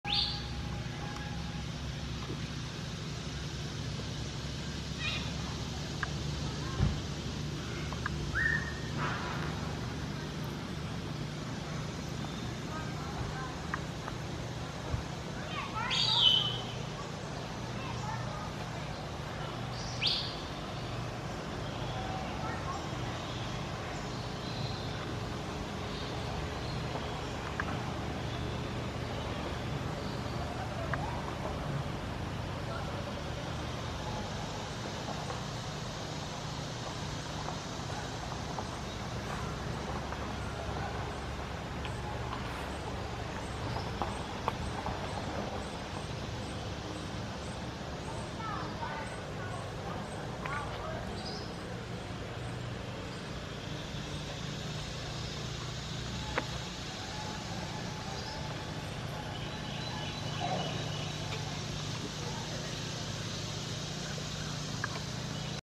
Star Gazer Lily, Peacock Statue & Bird Sounds by Bird Feeder in Peaceful Garden at Sylvan Heights
Star Gazer Lily, Peacock Statue & Bird Sounds by Bird Feeder in Peaceful Garden at Sylvan Heights Bird Park 2016! Scotland Neck, NC